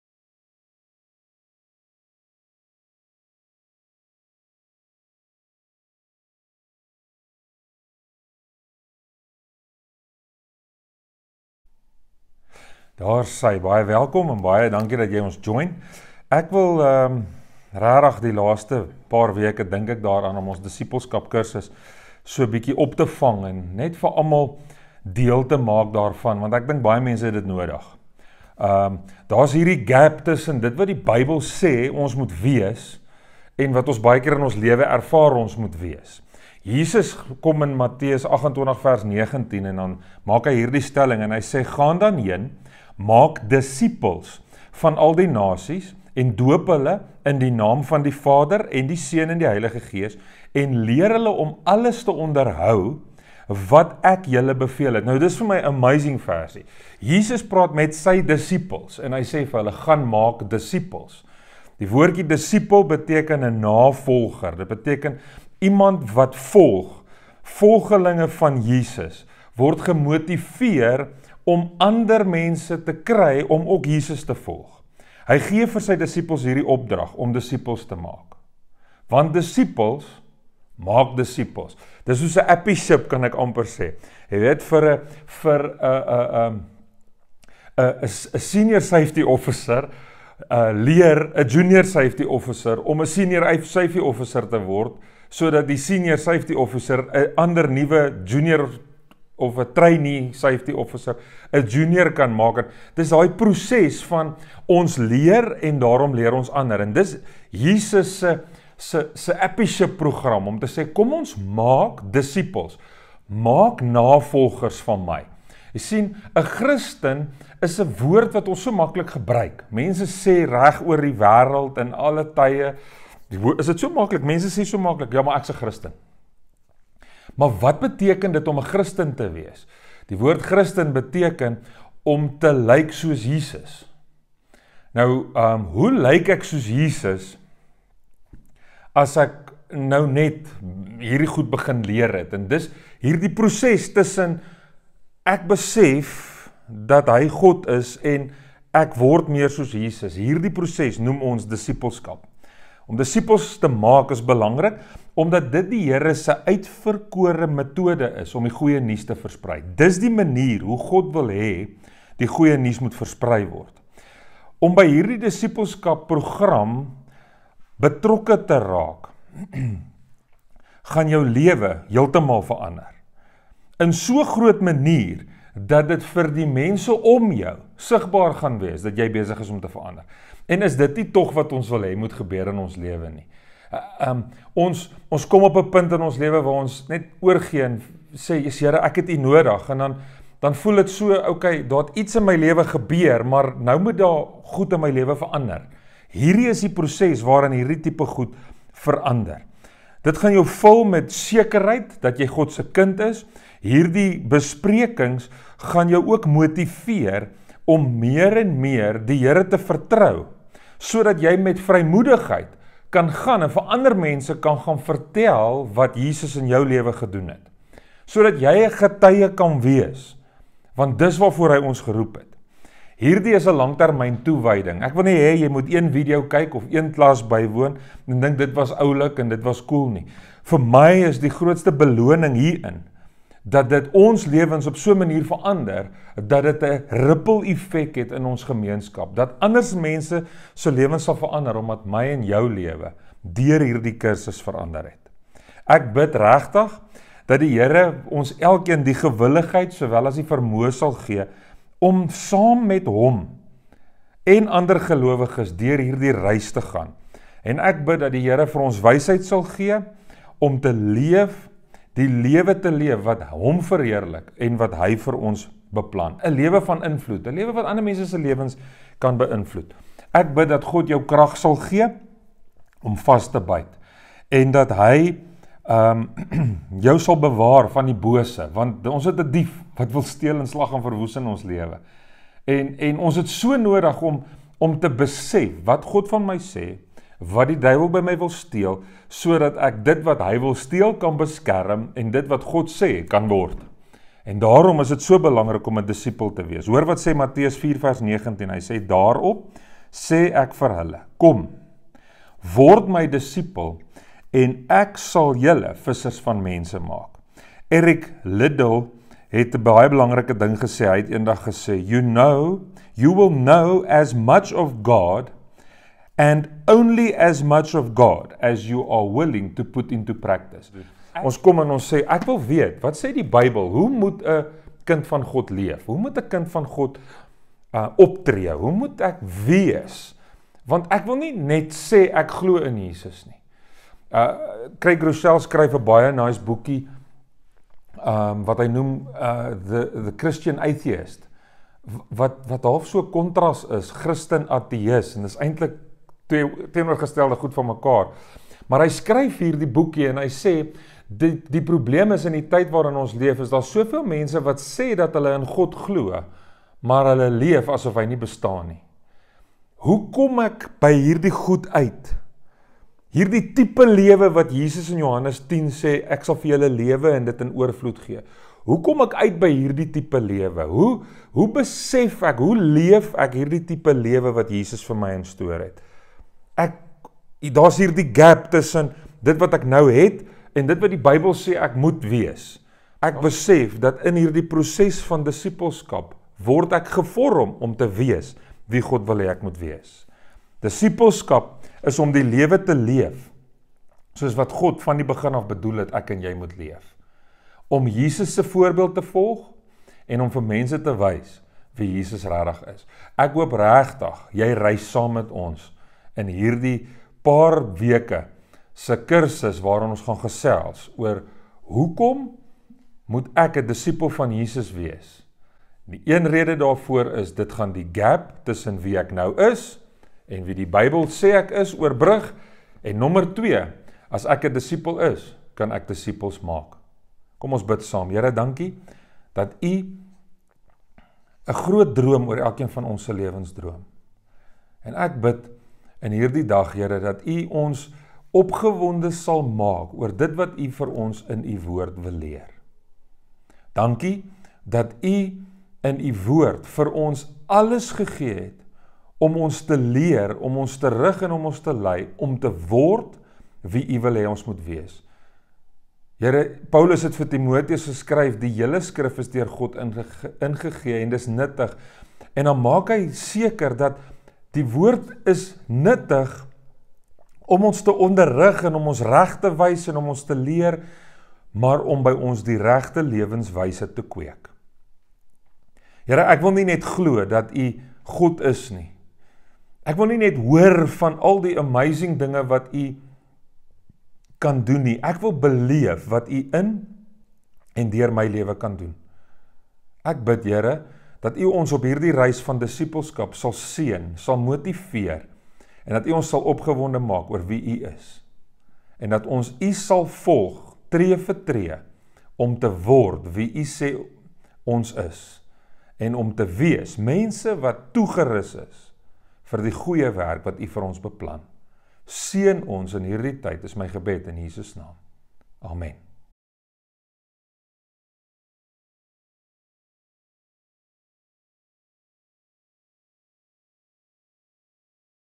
Dissipelskap 1 Inleiding
Hoekom moet ons dissipels van Jesus wees en dan dissipels maak?